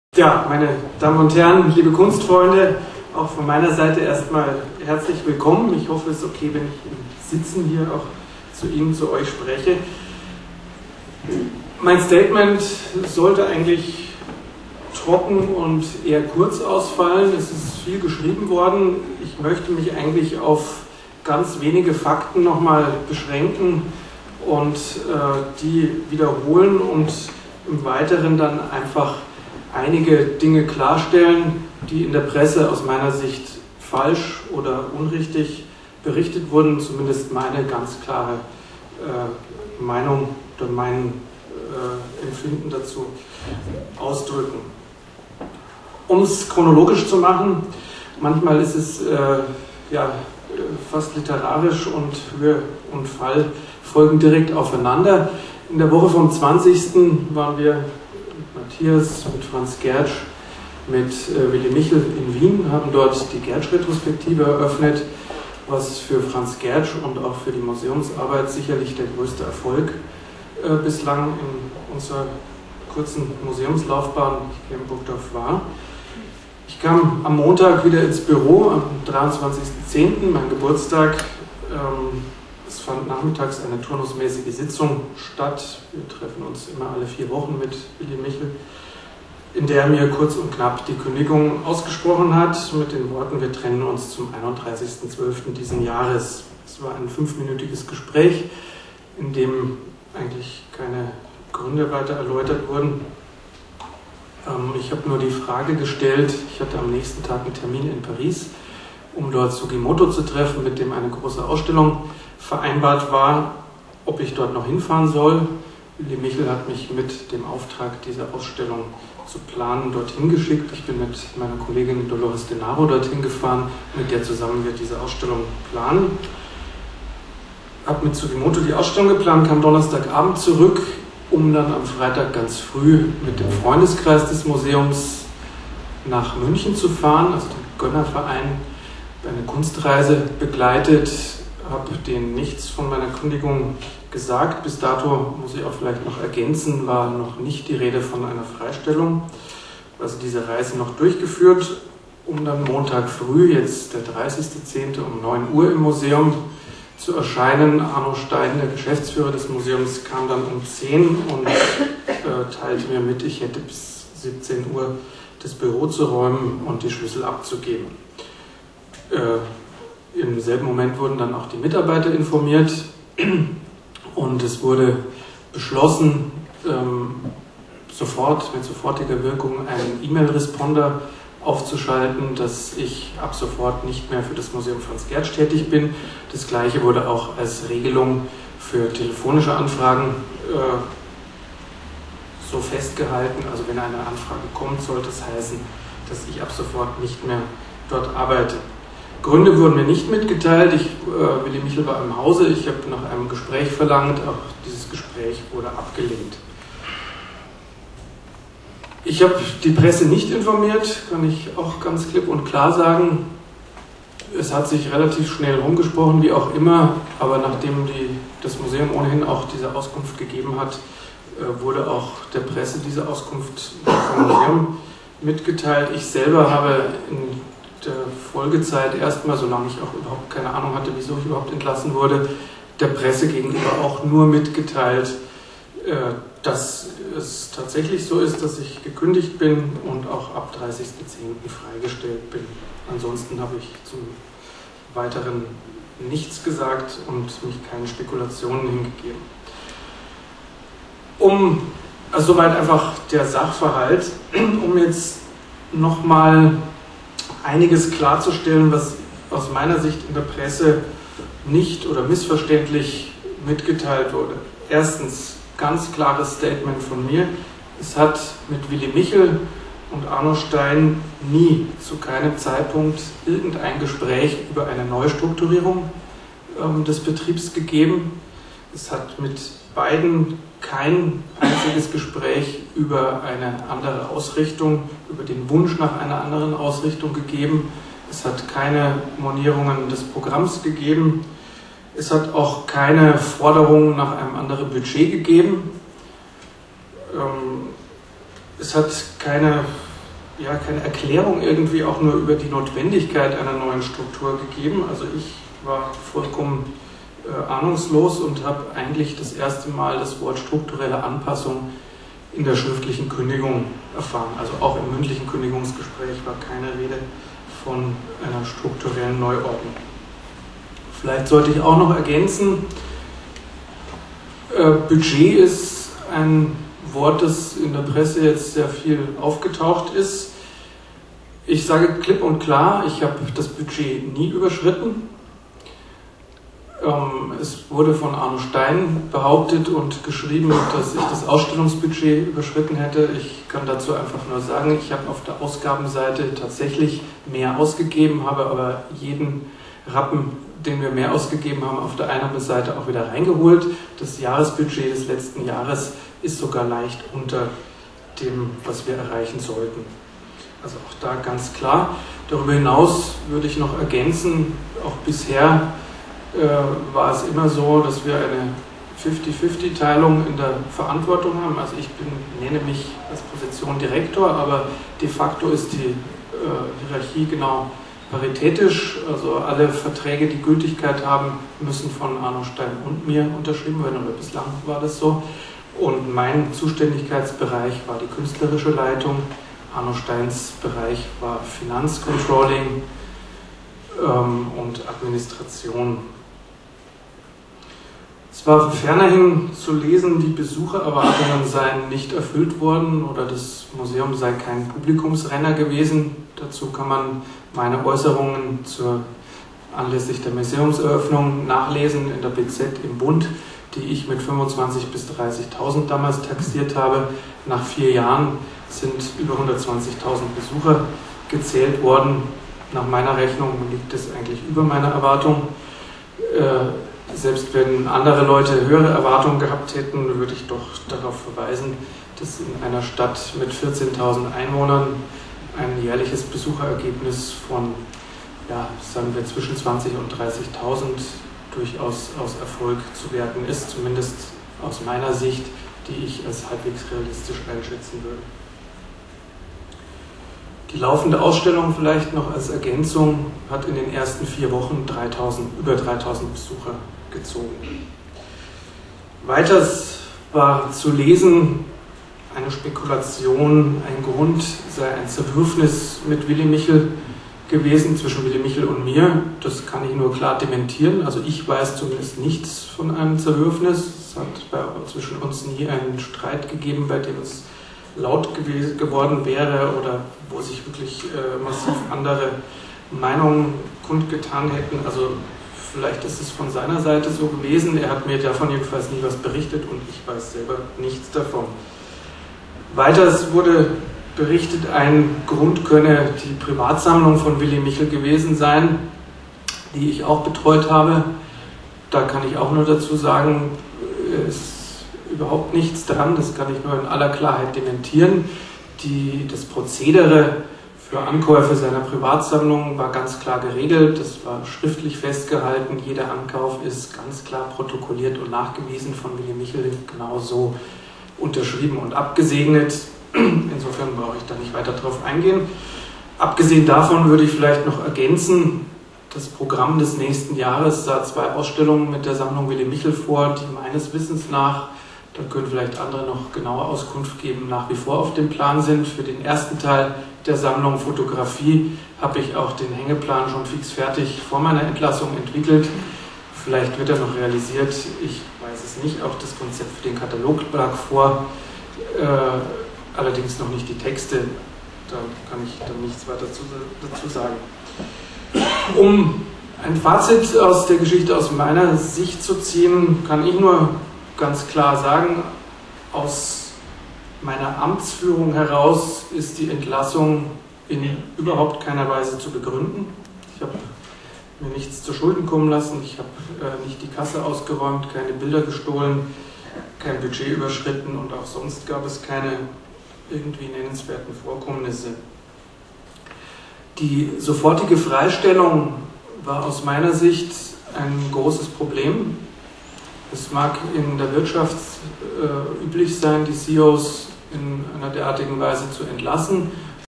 gertschmuseum rücktritt
reinhard spieler nimmt stellung zu seiner abrupten freistellung als direktor des museum franz gertsch in burgdorf.

tacheles-podiumsdiskussion der visarte bern zum thema "macht der medien" vom 21.11 im